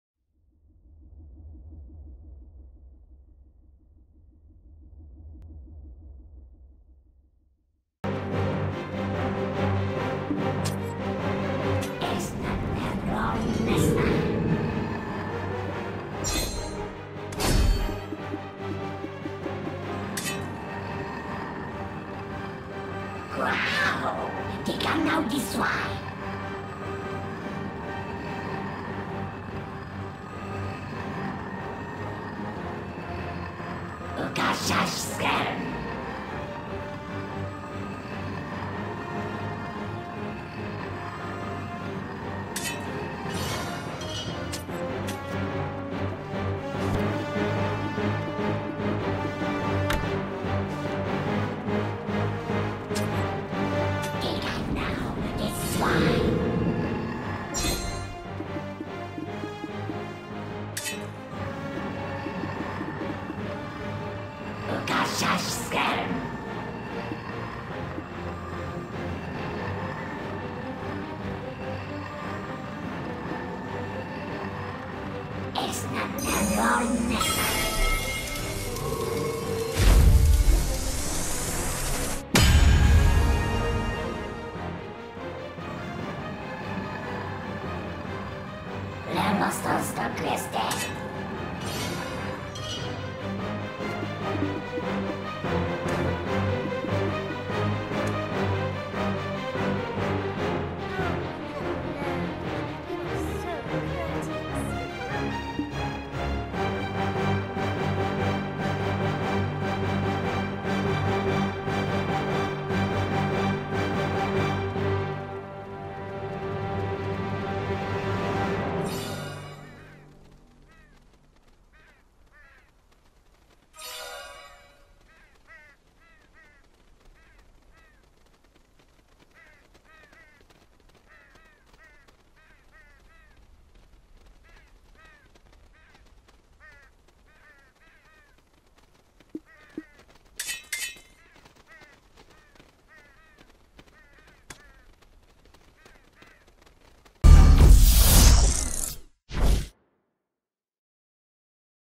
Blackstar Weapon Enhance*
Trying to get to DUO grade on the 1st attempt bcos the item used for enhancing is so hard to get!! At least on DUO grade, next enhancing can use cron stones to protect it from dropping.